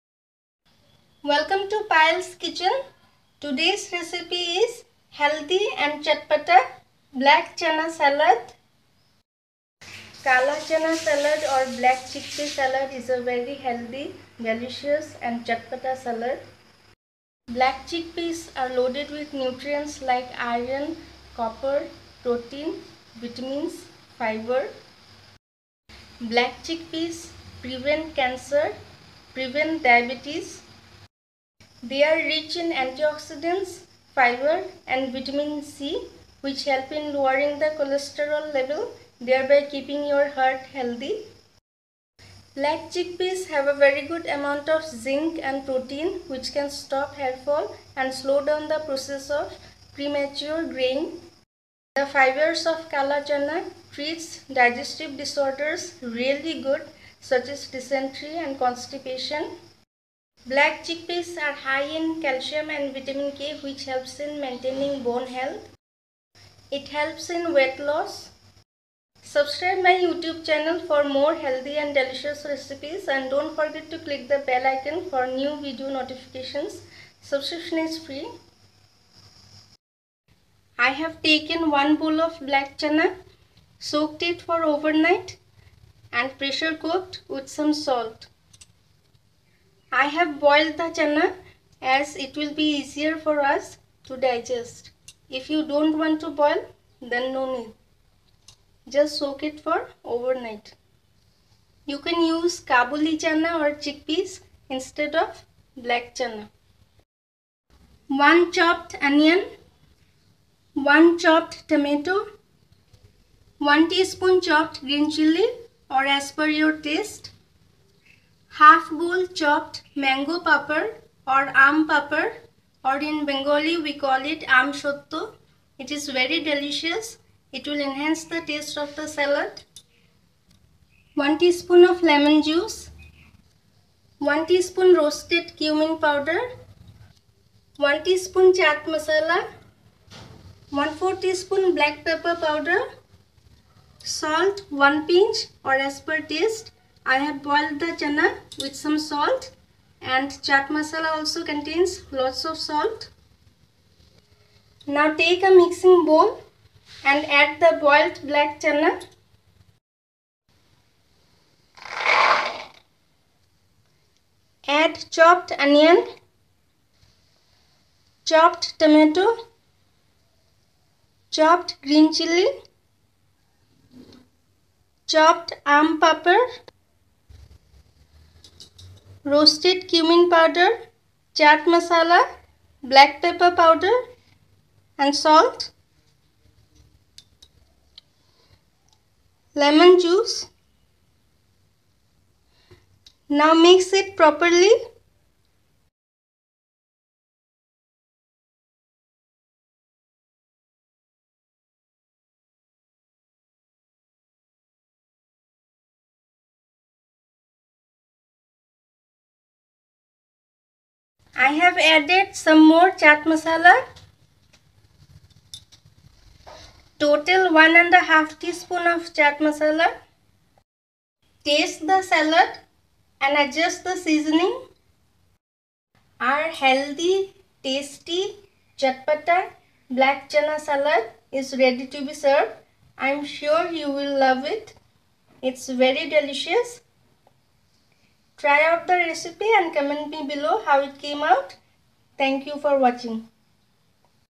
Black chickpeas salad recipe | Kala chana salad recipe | Healthy and chatpata black chickpeas salad
Black chickpeas salad or kala chana salad is a very healthy, delicious and chatpata salad. Black chickpeas are loaded with nutrients like iron, protein, copper, fibre, vitamins. Black chickpeas prevents diabetes. Prevents cancer. They are rich in antioxidants, fibre and vitamin C which helps in lowering the cholesterol level thereby keeping your heart healthy. Black chickpeas are high in calcium and vitamin K which helps in maintaining bone health. Black chickpeas have a very good amount of zinc and protein which can stop hairfall and slow down the process of premature greying. Black chickpeas treat digestive disorders really good such as dysentery and constipation. It helps in weight loss.

Ingredients :-
1. Black chickpea / Kala chana soaked for overnight and then pressure cooked - 1 bowl of raw kala chana
2. Chopped onion - 1
3. Chopped tomato - 1
4. Chopped green chilli - 1 teaspoon or as per your liking
5. Chopped mango papad / aampapad / aamshotto - 1/2 bowl
6. Roasted cumin powder - 1 to 1 and a 1/2 teaspoon
7. Chaat masala - 1 and a 1/2 teaspoon
8. Black pepper powder - 1/4 to 1/2 teaspoon
9. Salt / black salt - 1 pinch